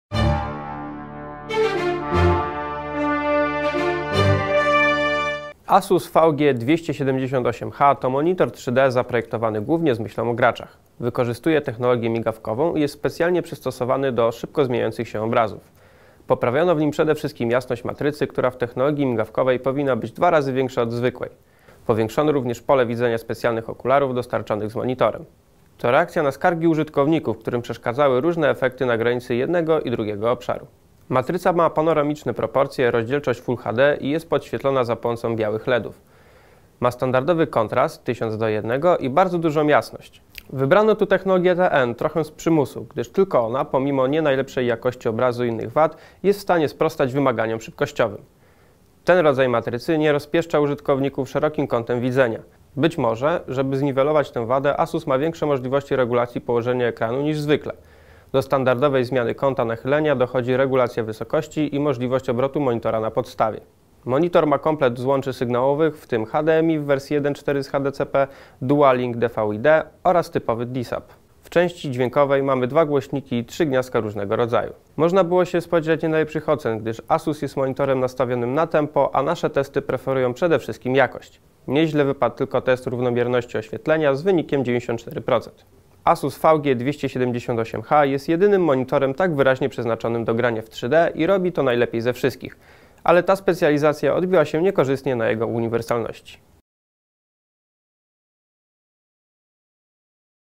Asus VG278H - test PC World
Asus VG278H jest monitorem 3D zaprojektowanym dla graczy. Wykorzystuje technologię migawkową i jest specjalnie przystowany do szybkozmieniających się obrazów. Ma jasność przekraczającą 300 cd/m2 , 120Hz i czas reakcji 2 ms. Matryca wykonana w technologii TN ma rozdzielczośc 1920x1080 jest podświetlana LED-ami. Monitor ma komplet złączy sygnałowych w tym HDMI w wersji 1.4 z HDCP, Dual-link DVI-D oraz typowy D-Sub.